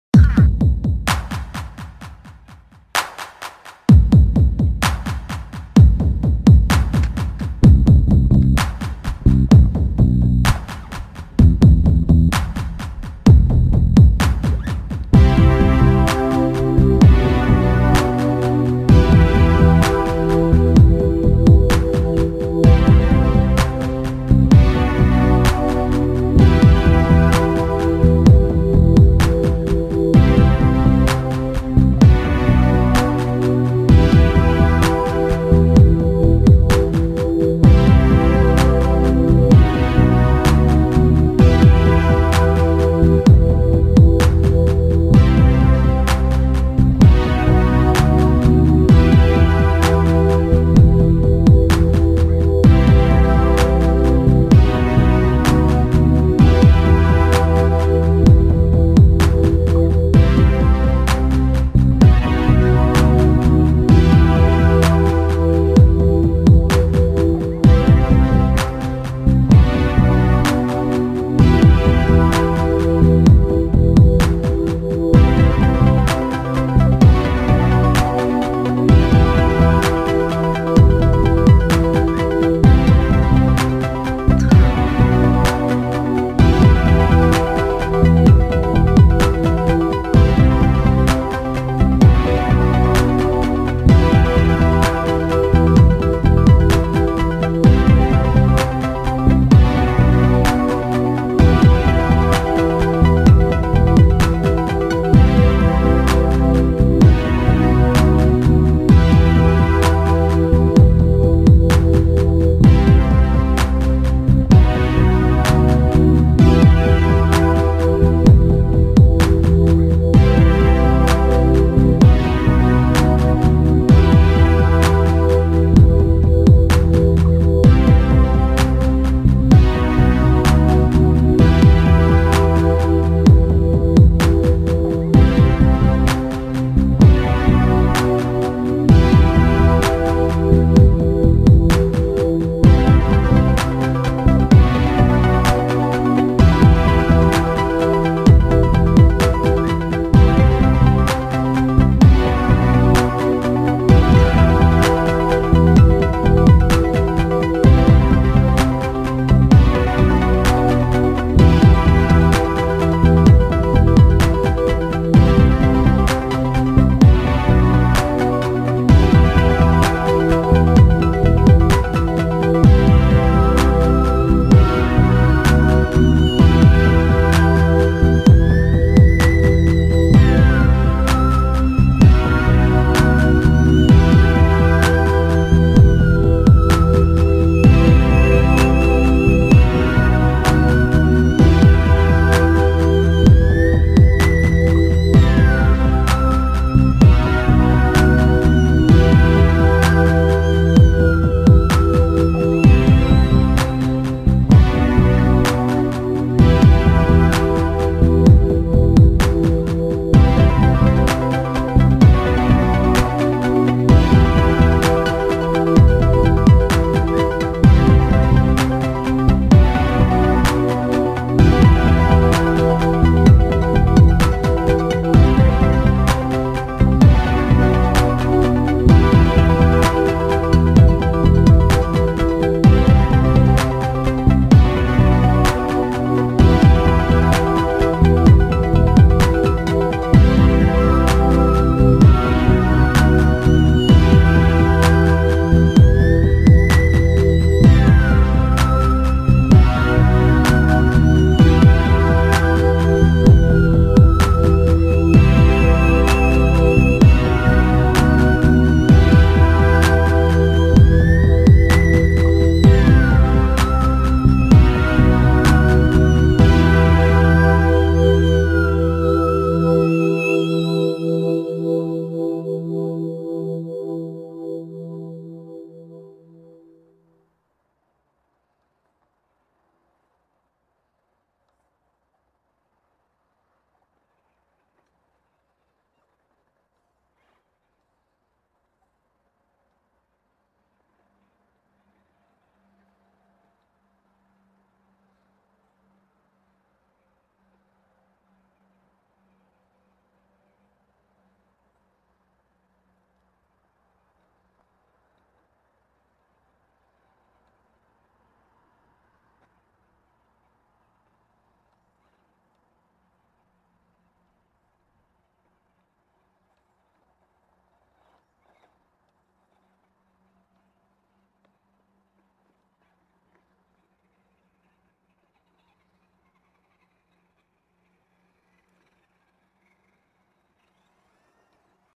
remo hobby 1093-st 1:10 honcho rc crawler , first test outdoor part1/2
test du 1093-st en exterieur voir ce qu'il a dans le ventre😁😉, ben á part son pont qui traine par terre(vive les ponts portique😌👌) et les pneux, sinon c'est plutot pas mal.